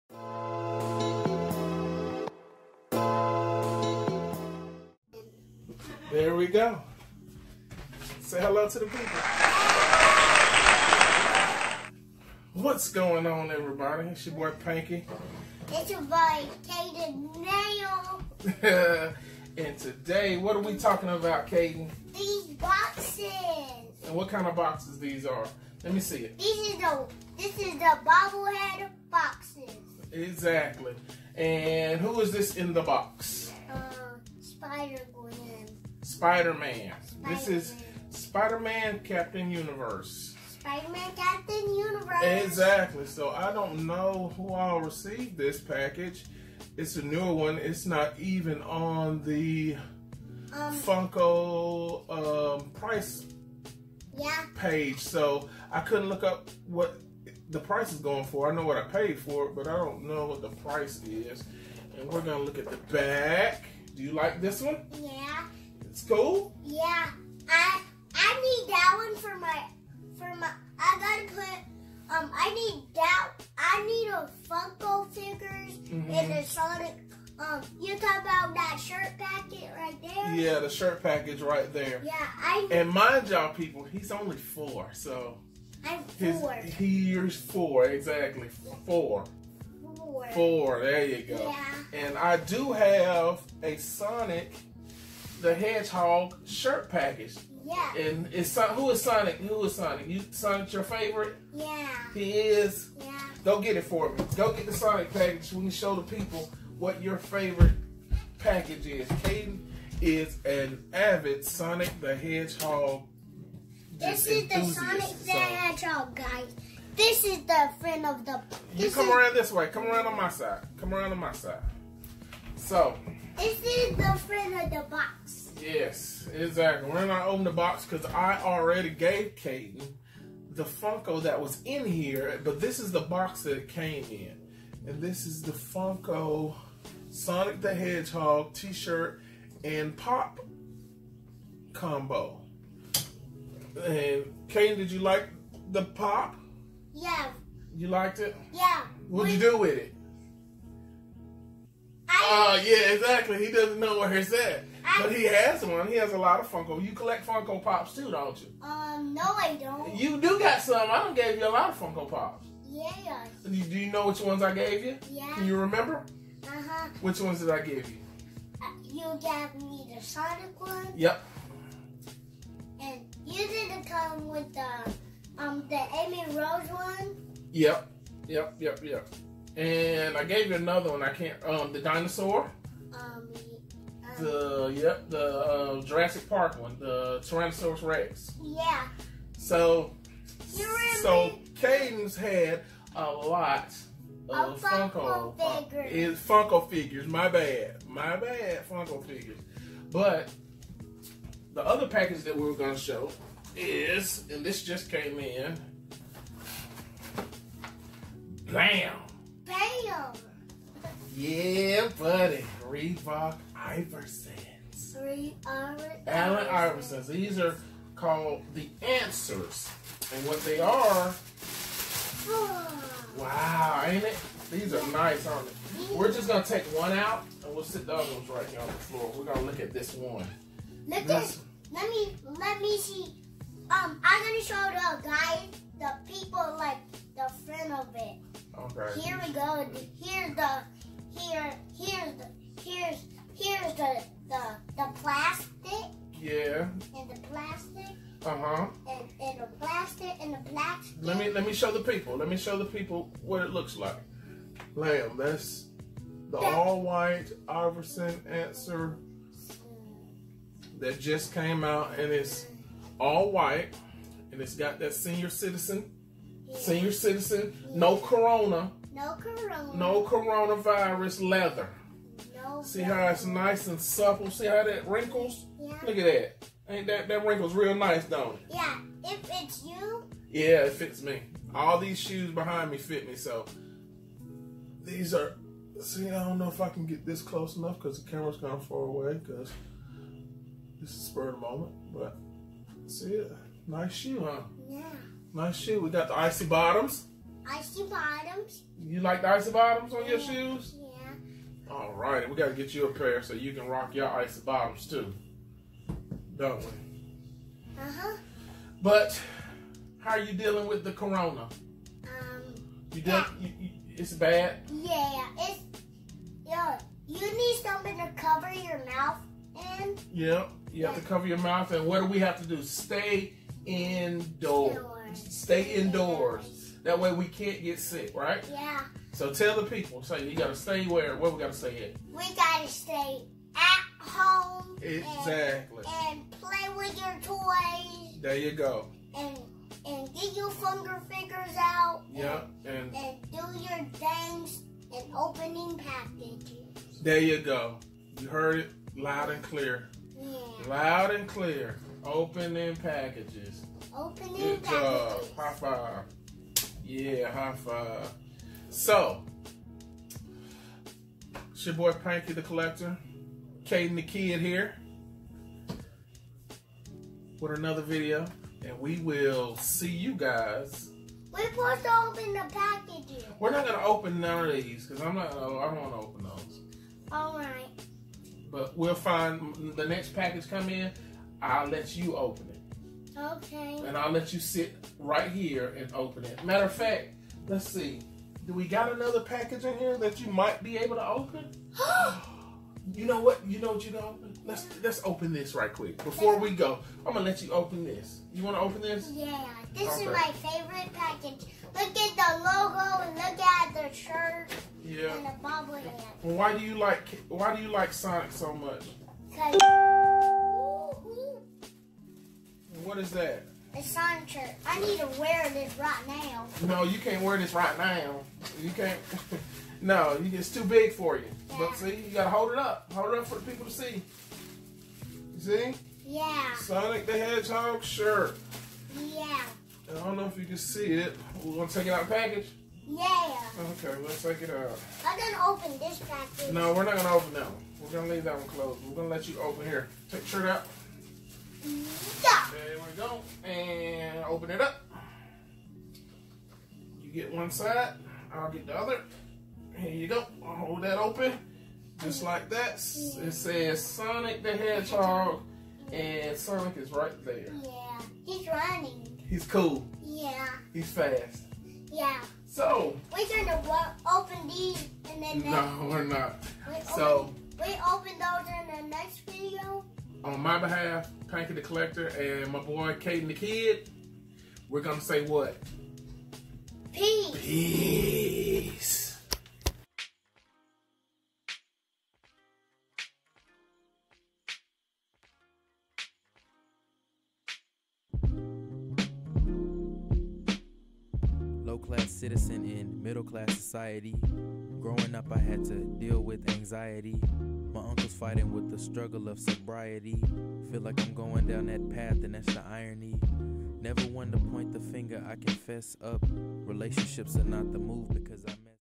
Funko/ Iverson Answer reveals wit Cayden tha Kid!!!
Showing Funko pop and Iverson Answer shoes with my grandson Cayden tha Kid.